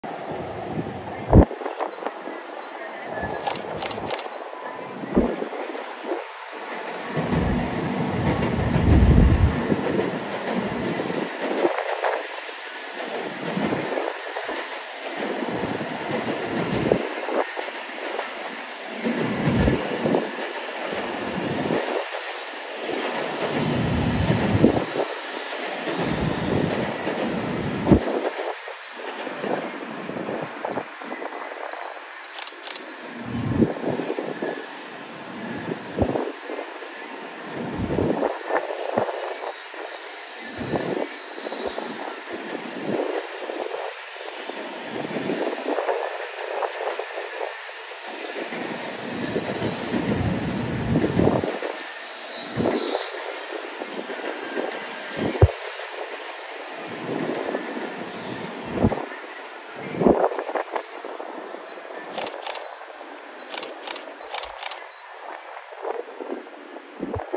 JR Central Shinkansen Meet! N700-1000 series "N700A" meet at Yurakucho Colline Garden
Shinkansen Meet! N700-1000 series "N700A" G33 meets G46 at Yurakucho Colline Garden
N700-1000 series "N700A" G33 made by Nippon Sharyo on 21 April, 2017
N700-1000 series "N700A" G46 made by Nippon Sharyo on 23 March, 2019